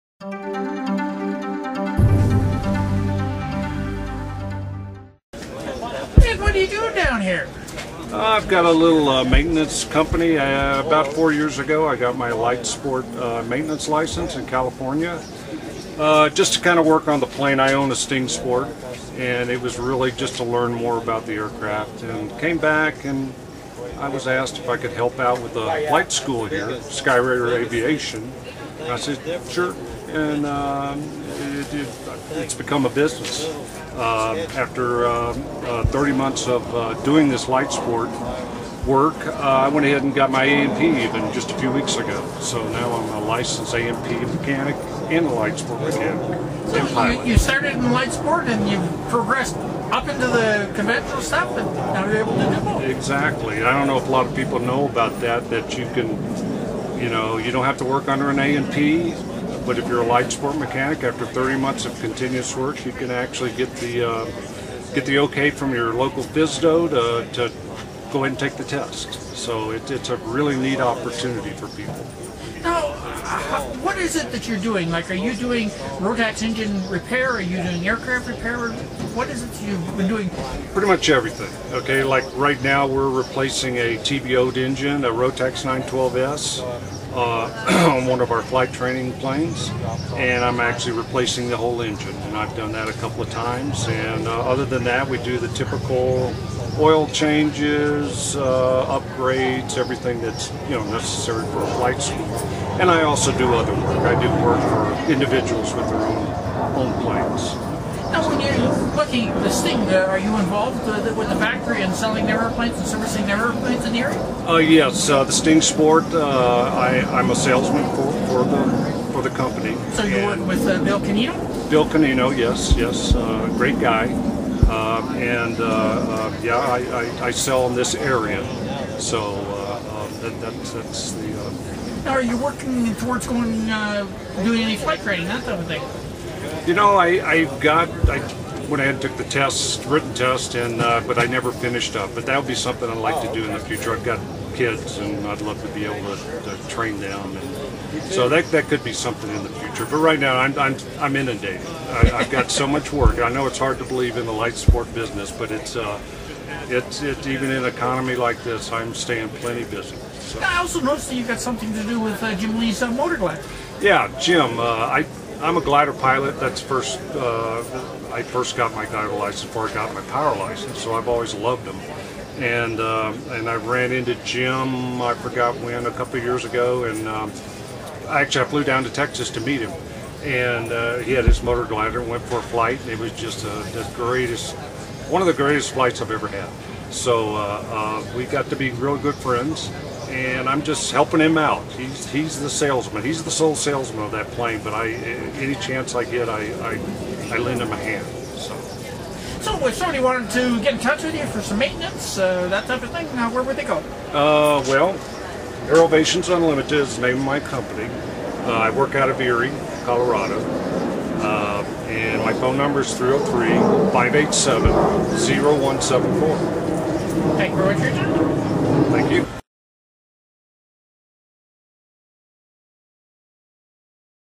Light Sport Aircraft Maintenance, Aerovations Unlimited, Erie Colorado.
- Light Sport Aircraft Maintenance, Aerovations Unlimited, Erie Colorado. Ed Babavec of Aerovations Unlimited talks to us about his Rotax aircraft engine and light sport aircraft repair facilities and about the Sting Sport light sport aircraft and the Phoenix Motor Glider, during the Rocky Mountain Light Sport Aircraft Expo at Front Range Airport Colorado.